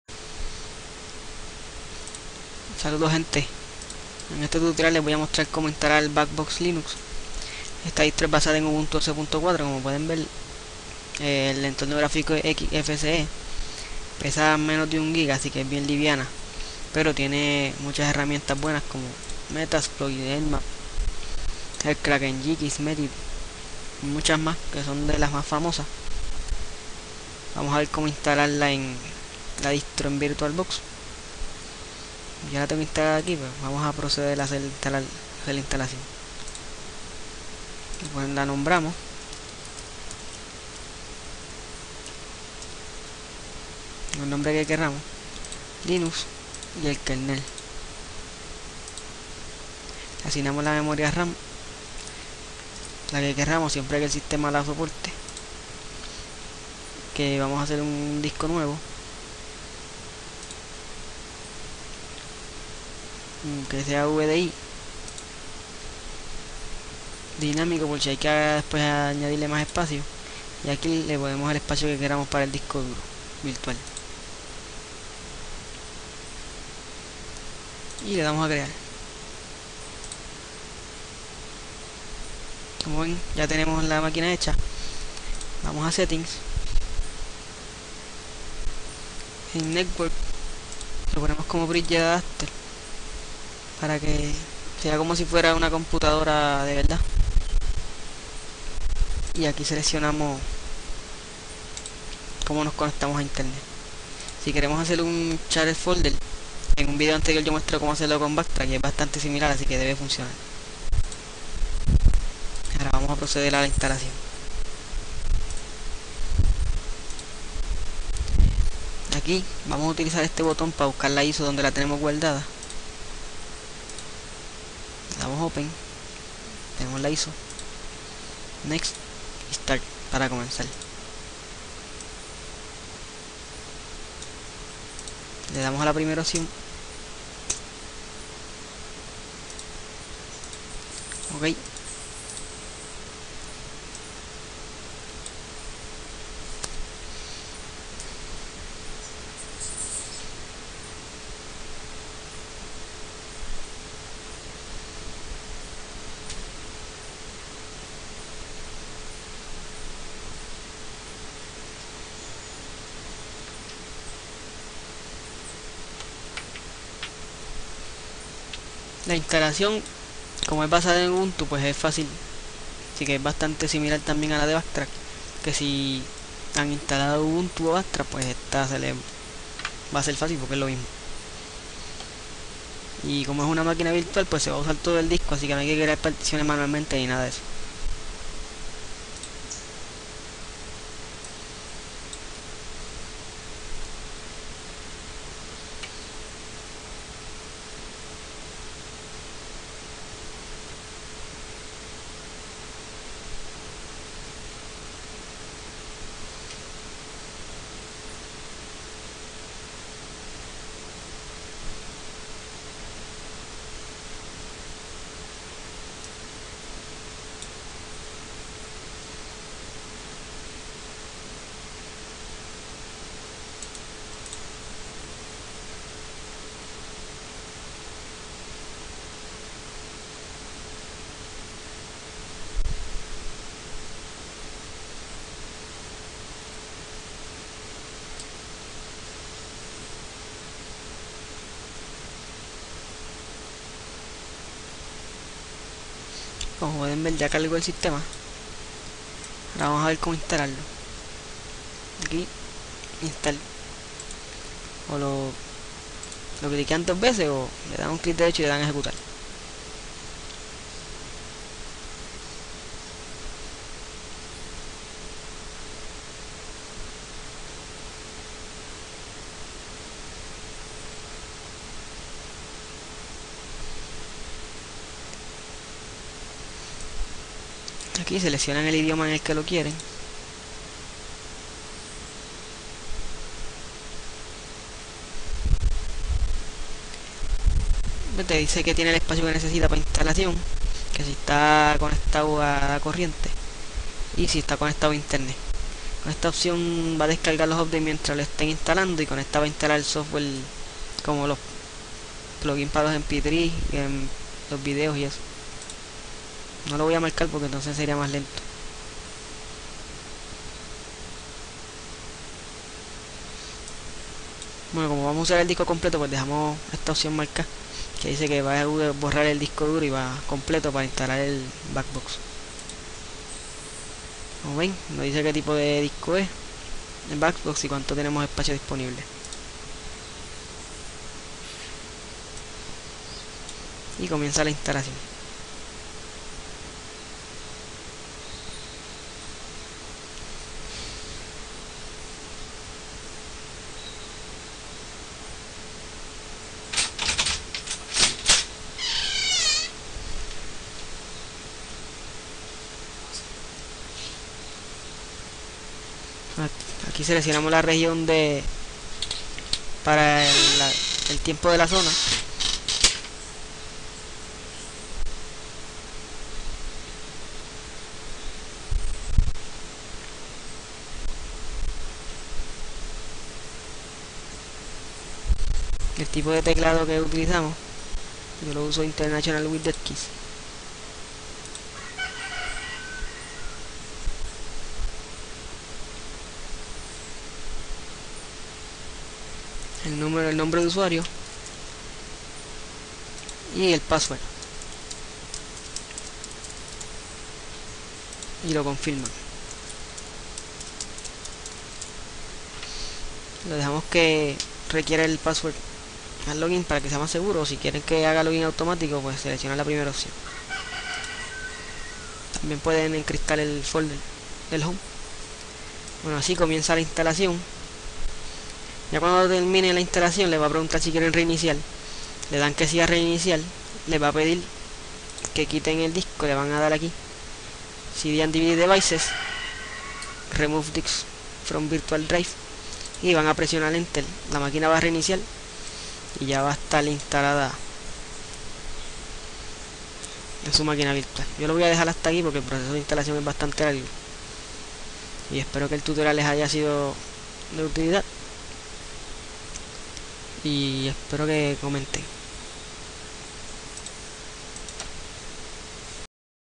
Como instalar BackBox en VirtualBox
como instalar BackBox, distro para pruebas de seguridad.